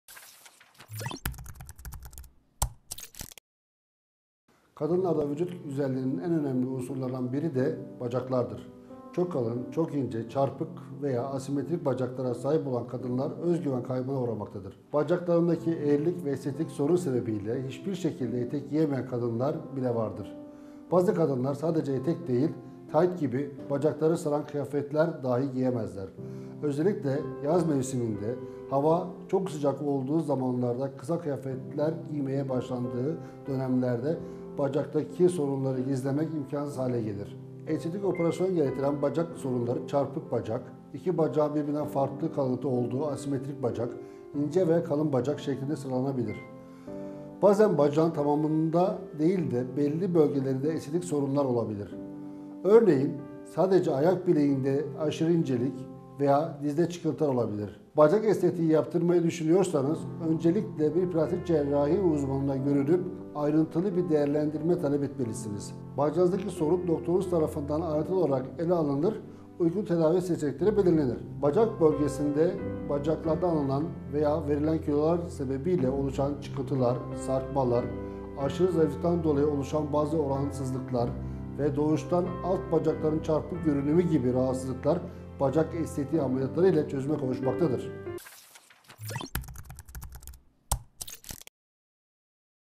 Bacak estetiği hangi durumlarda yapılır ?
Çok kalın, çok ince, çarpık veya asimetrik bacaklara sahip olan kadınlar özgüven problemi yaşamaktadırlar. Bacak estetiği düşünülüyorsa öncelikle bir plastik cerrah uzmanına görünüp ayrıntılı bir değerlendirme talep edilmelidir.

Op. Dr. Ali Cemal Yılmaz
Plastik ve Estetik Cerrahi Uzmanı